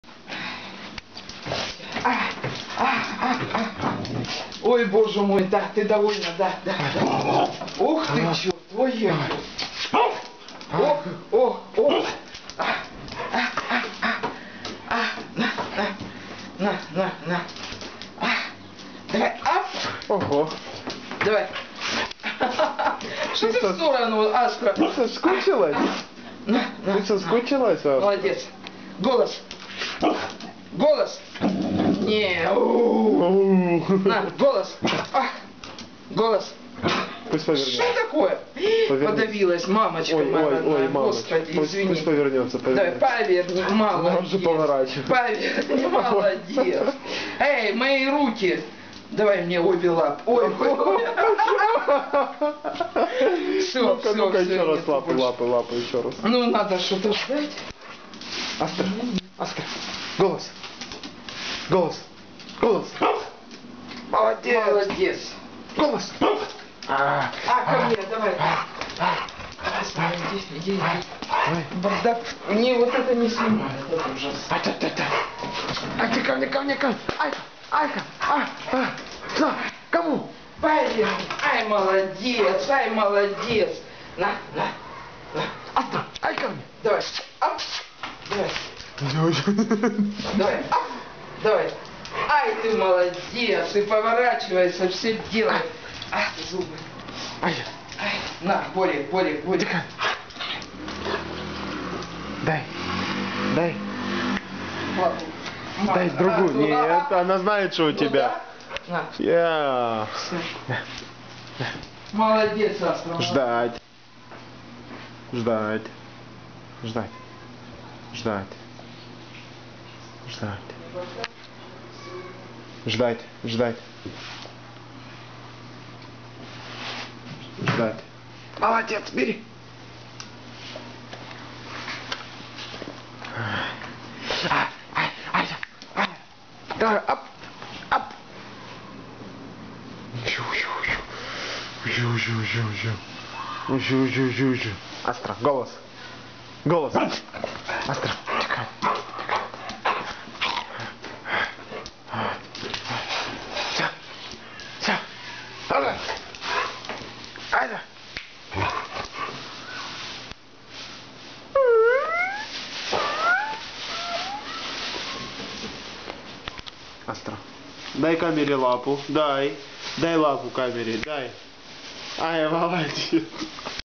Astra Meets Fer Friends :)
We were shopping, we were out for like 2 hours, but as you might know the dog doesn't rationalize that you were out for 5 minutes or 10 hours, The Dog simply happy to meet you whenever you come back !